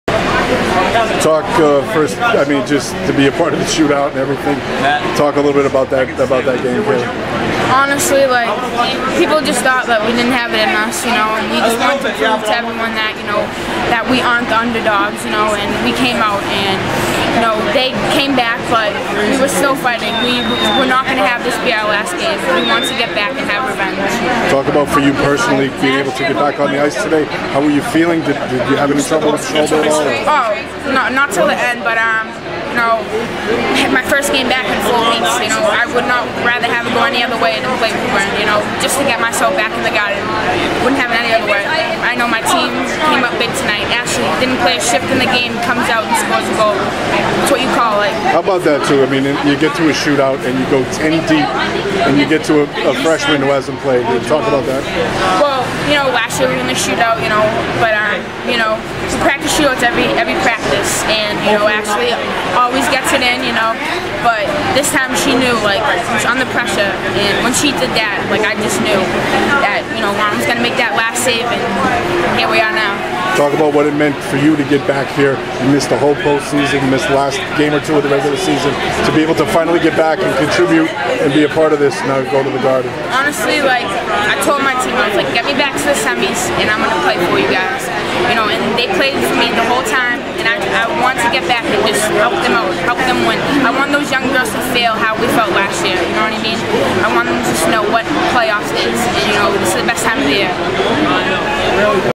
St. Mary's F Kaleigh Finigan
The senior returned after missing four weeks with a shoulder injury to help the Spartans defeat Woburn, 3-2 in a shootout, in the Div. 1 girls hockey state semifinals.